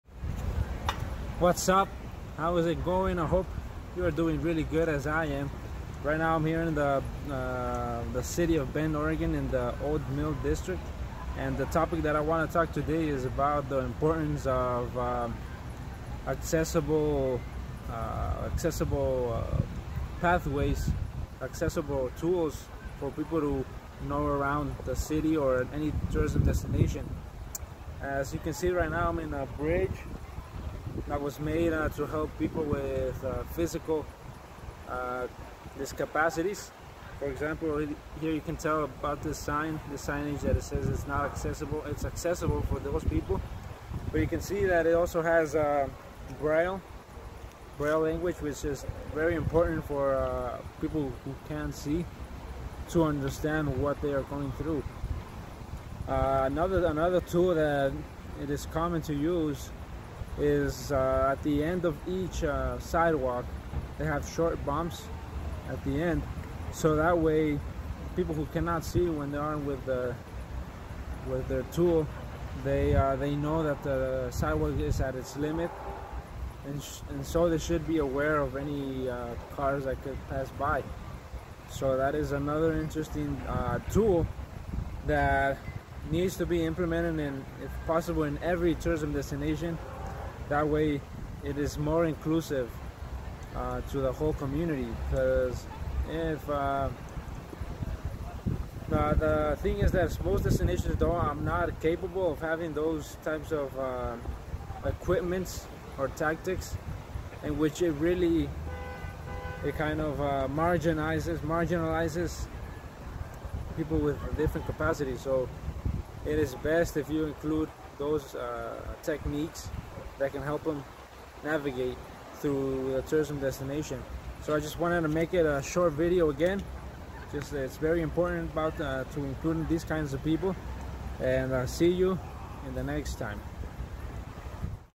The Importance Of Inclusive Tourism In A Destination
Why should there be travel locations with certain access for people with different capabilities?

It is a really good question to ask yourself...

And it is a really complex circumstance to be in.
The mission of Tourism is to bring more people together, since it offers a way for the community to expose their best self and it incentivises external people to take care and enjoy what they see.

For people who wonder why, hopefully this video will help out

What are your takes on this?